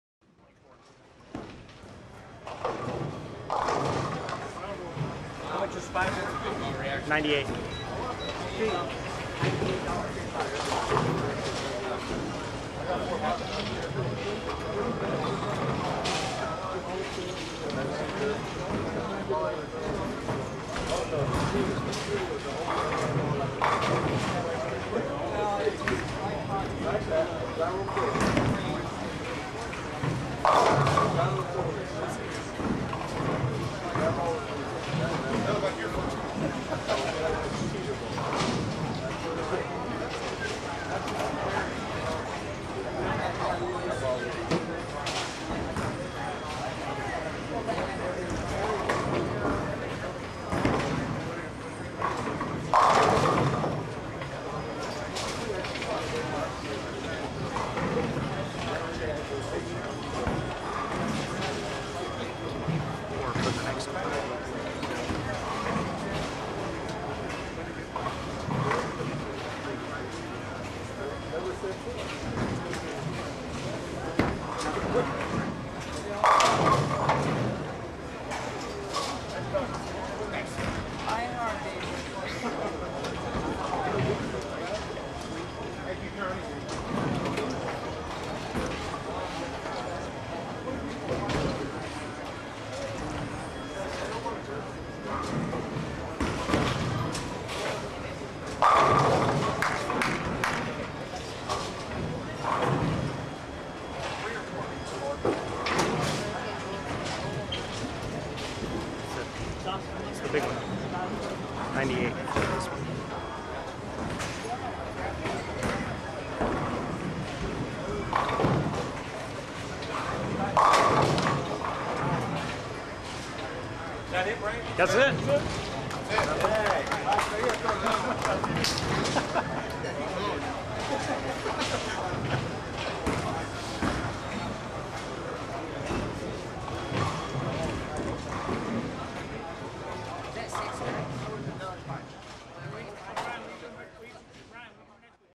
Steve Warren Bowling for $98 (Nov 8, 2010) - Strike Run
Bowling for a top prize of $98 for 5 consecutive strikes. November 8, 2010.

to learn more about the Strike Run Series.